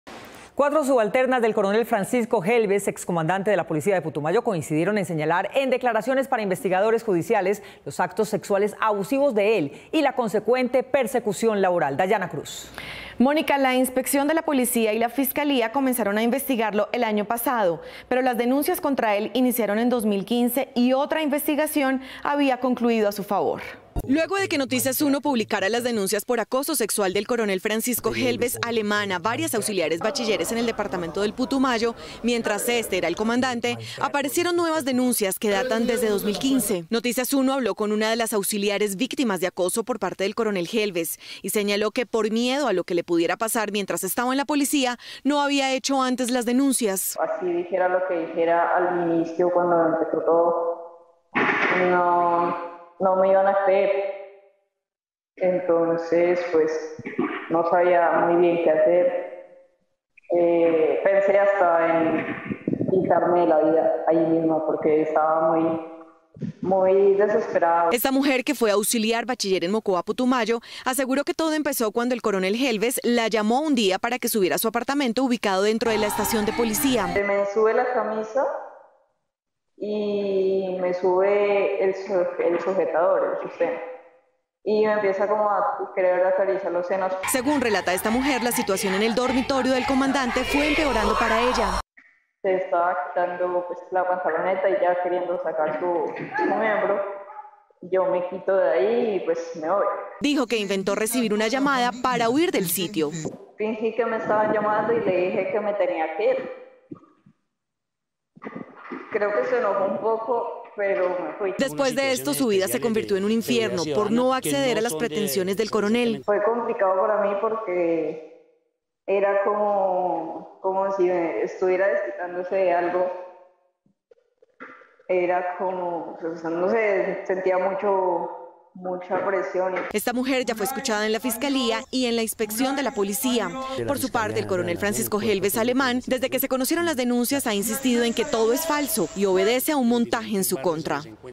Otras víctimas revelan abusos sexuales de su superior, un coronel
Cuatro subalternas del coronel Francisco Gelves, excomandante de la Policía de Putumayo, coincidieron en señalar, en declaraciones para investigadores, judiciales, los actos sexuales abusivos de él y la consecuente persecución laboral.